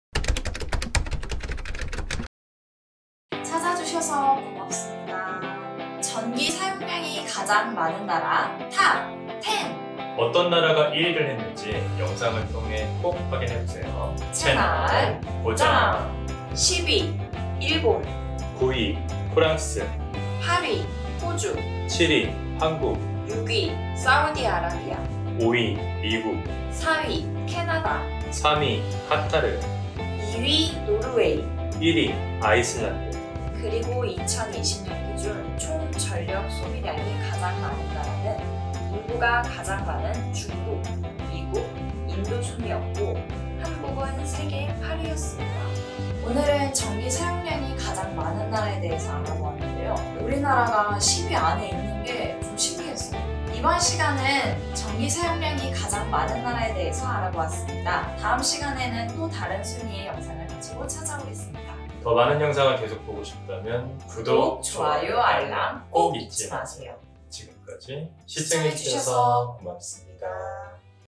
전기 사용량이 가장 많은 나라 탑10
어떤 나라가 전기사용률이 가장 많을까?